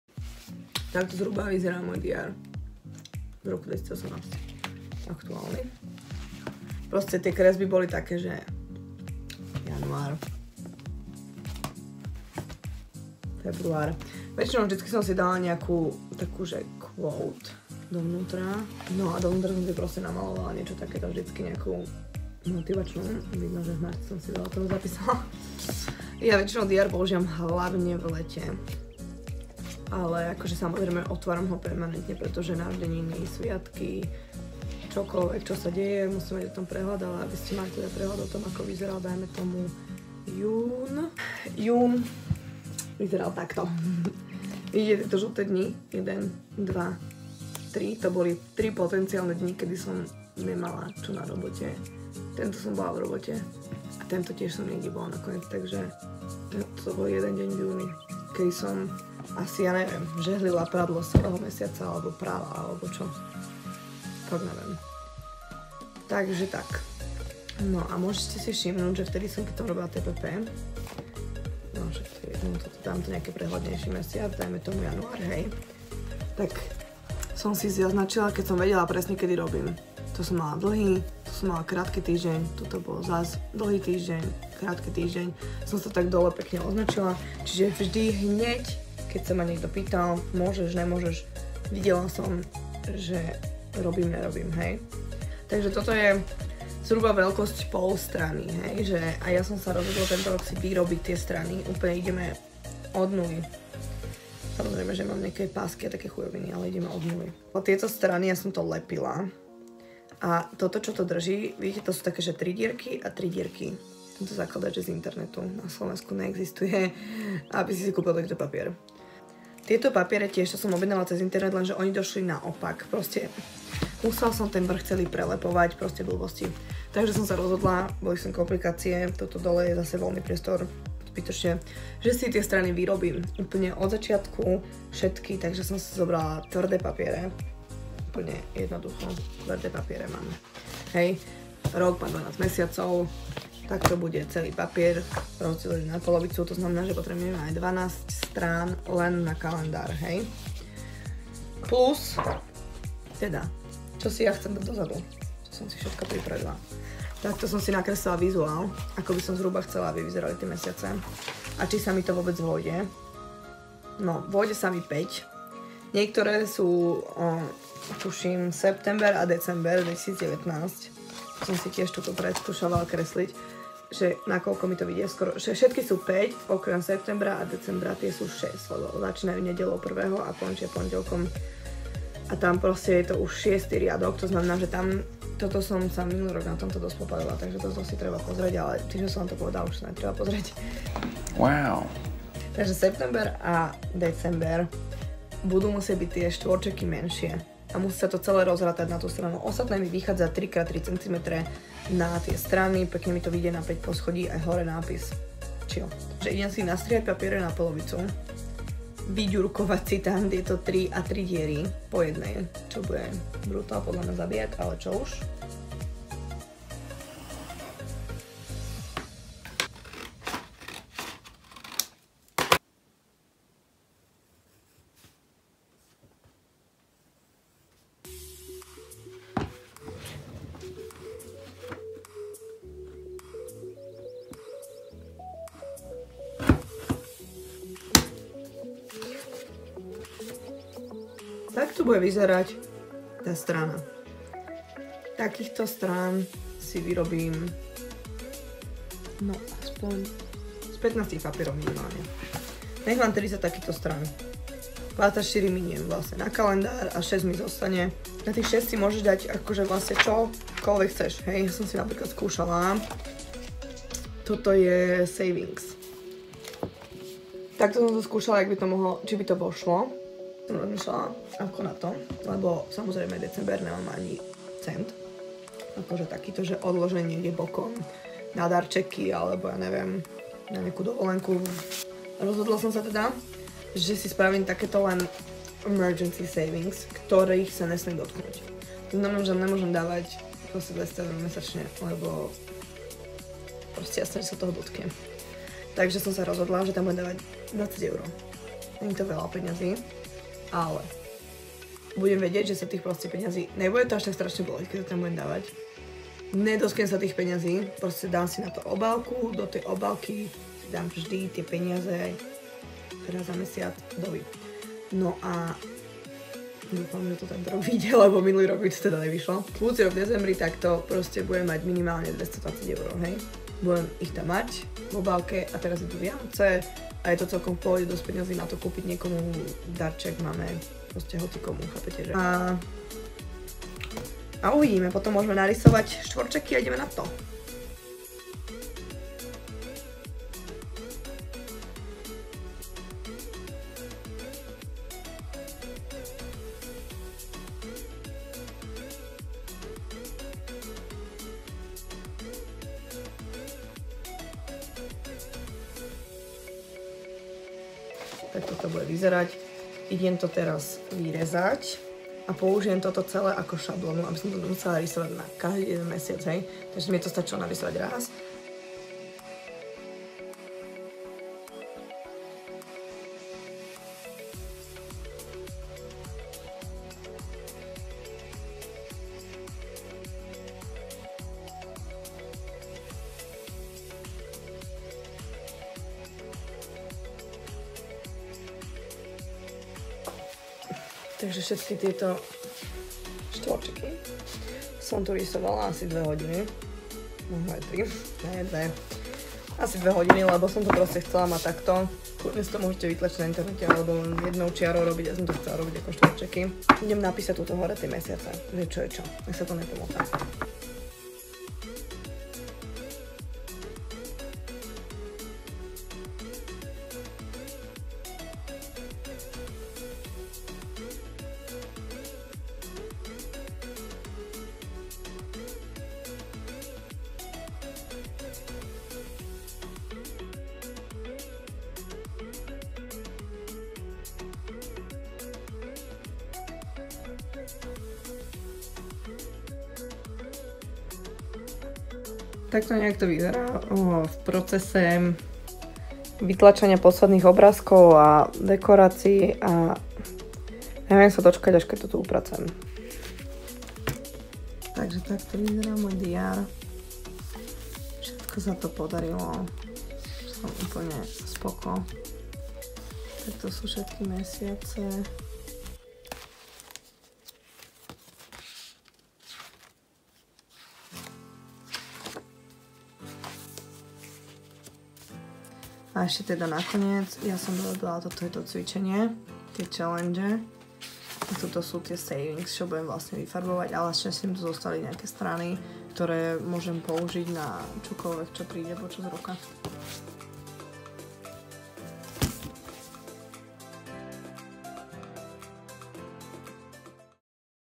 Ako si vyrábam vlastný diár na rok 2019
Rozhodla som sa vyrobiť si svoj vlastný diár už pred 3 rokmi, ten prvý sa na tento posledný ani zďaleka nepodobal, ale každý mi bol k srdcu blízky, pretože som ho vyrábala sama! Diár je našou súčasťou pocelý rok a tak pokiaľ sa ti tento nápad páčil, určite neváhaj a vyrob si svoj vlastný diár/planner/ bullet journal aj ty! Nezabudni mi poslať tvoj výtvor na môj insta @redbullsvodkou !! :)